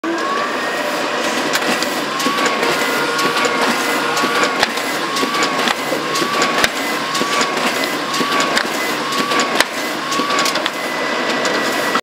Risograph Printing HD
Some Riso-graph printing we did at Camberwell College of Art. I am currently studying BA Graphic Design and we were asked to design a book based on an essay written by Vilem Flusser. We chose to print our books with the Risograph printer because it gave a nice texture to the images and the one tone colour made the images stand out more.

Picture credit to Alex Herrera